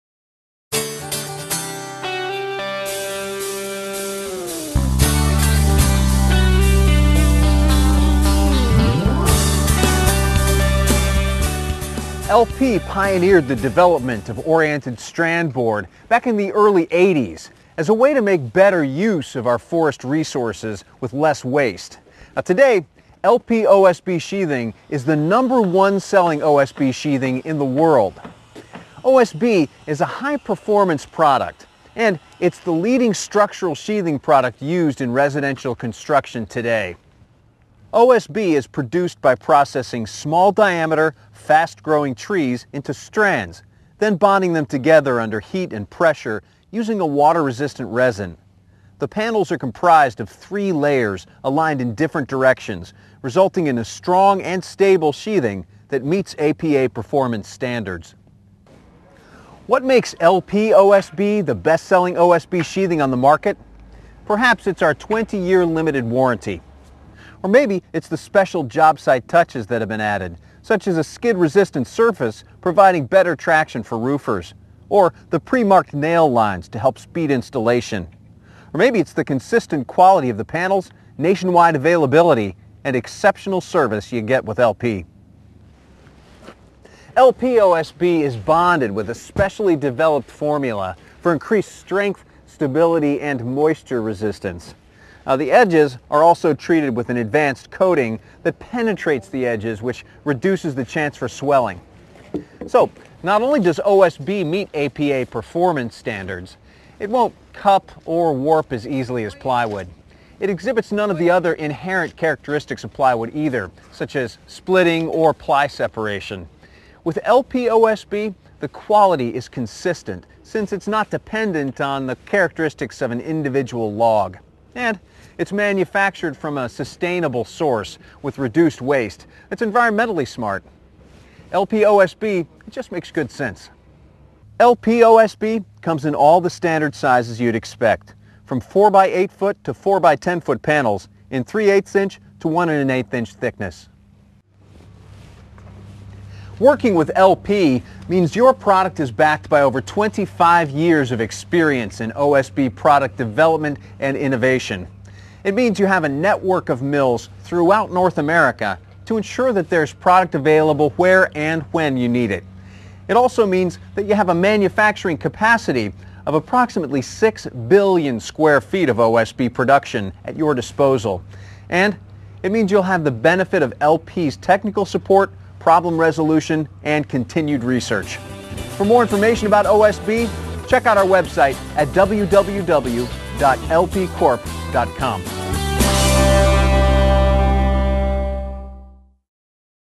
LP OSB Sheathing
Engineered as a cost-effective, high performance alternative to plywood, LP® OSB® Sheathing - the No. 1 selling sheathing in the world - delivers all the advantages of plywood with less risk of cupping, warping, splitting and delamination.